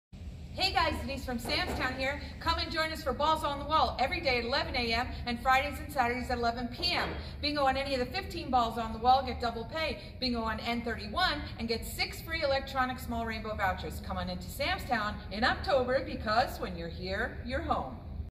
October Balls on the Wall
🍂Who's ready for Fall Bingo 𝗕𝗮𝗹𝗹𝘀 𝗼𝗻 𝘁𝗵𝗲 𝗪𝗮𝗹𝗹?  Daily at 11am AND Friday & Saturday at 11pm in October.🍂

Bingo on any of the 𝟭𝟱 Balls on the Wall, receive DOUBLE PAY!! Bingo on 𝗡-𝟯𝟭, receive SIX FREE electronic small rainbow vouchers!